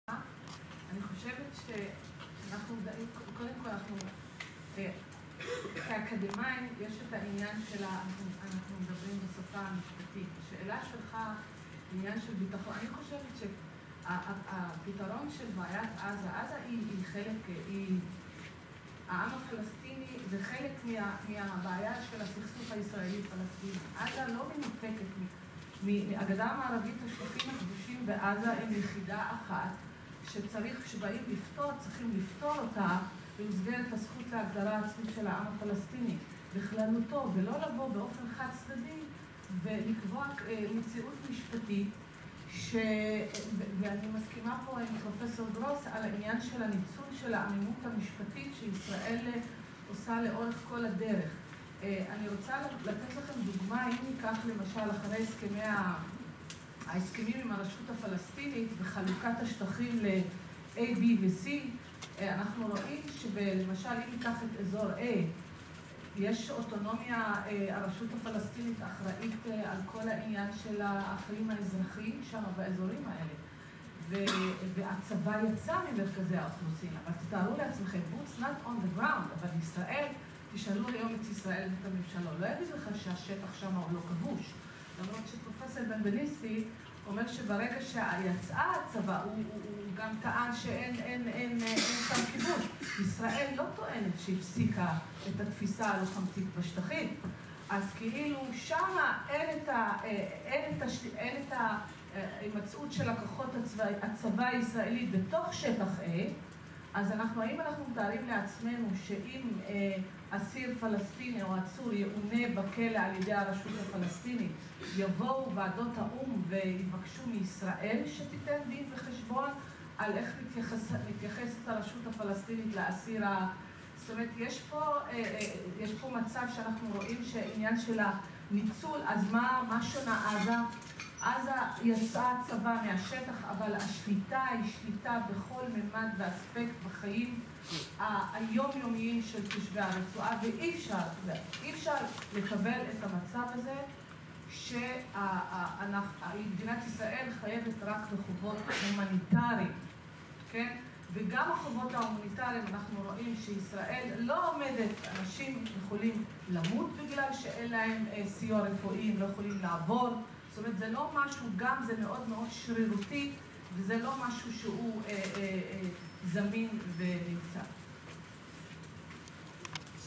תגובה I ד״ר האלה ח׳ורי־בשאראת I יום עיון "עזה במשפט" בעריכת כתב העת ״עיוני משפט״
🎬 לצפייה בכל סרטי הכנס 📹 תגובה I ד״ר האלה ח׳ורי־בשאראת I הקריה האקדמית אונו I קמפוס חיפה
שולחן עגול — מעמדה המשפטי של רצועת עזה
יום עיון "עזה במשפט" בעריכת כתב העת ״עיוני משפט״ ( غزة والقانون  يوم دراسي)
הפקולטה למשפטים I ע״ש בוכמן I אוניברסיטת תל אביב
מרכז מינרבה לזכויות האדם I הפקולטה למשפטים ע״ש בוכמן I אוניברסיטת תל אביב
פישר בכר חן וול אוריון ושות' - FBC & Co I Fischer Behar Chen Well Orion & Co